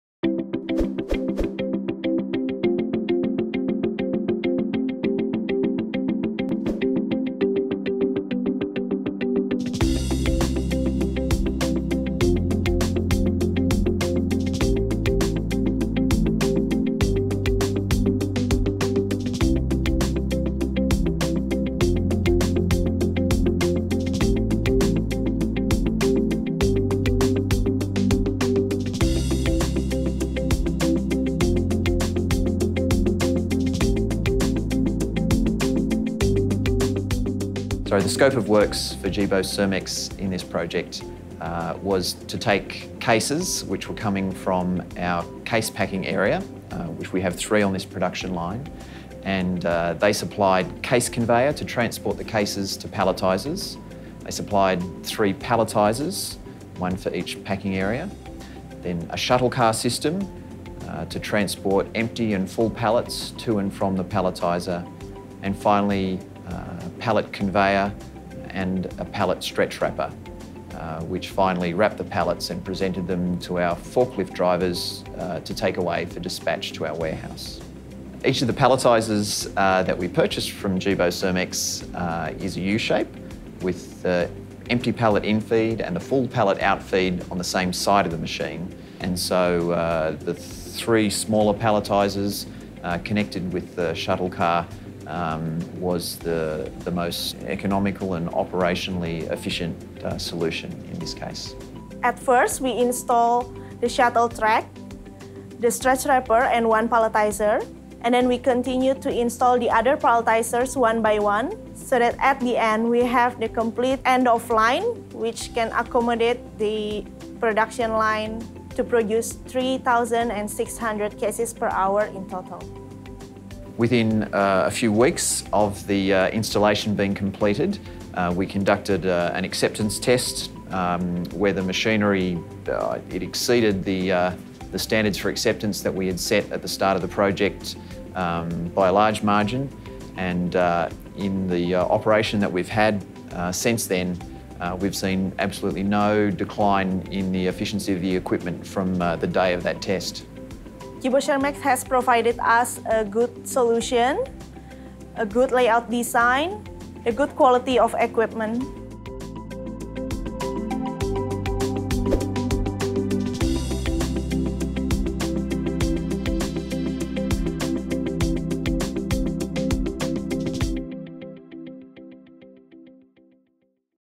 Coca-Cola Indonesia testimonial - Episode 2
The second episode of our Coca-Cola Indonesia testimonial on our End of Line expertise.